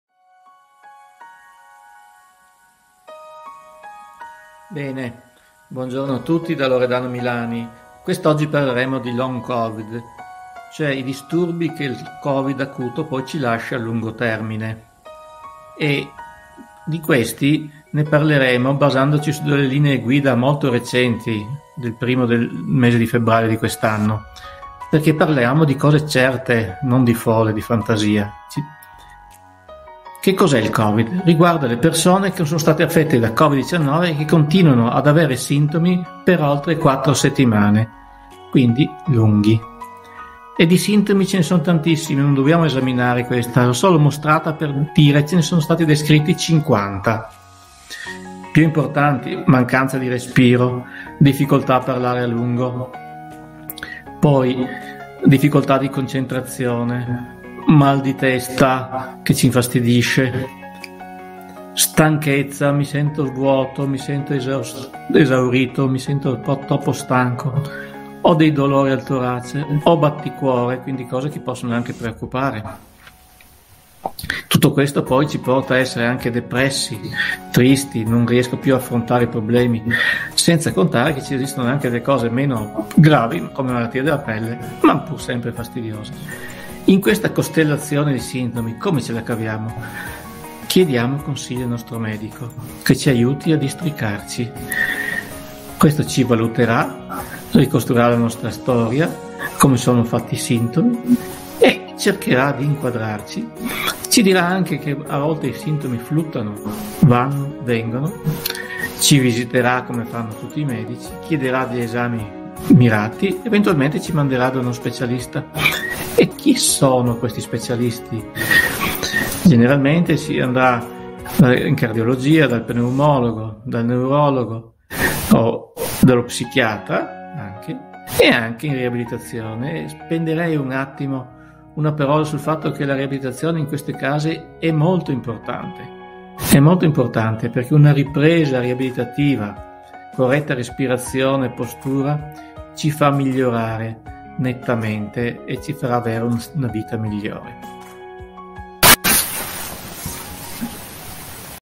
SINDROME DA LONG COVID - DR. MILANI
Sindrome da Long Covid.
Più di 50 sintomi permangono anche dopo un mese, dopo aver contratto la malattia e cosa si può fare per alleviarli.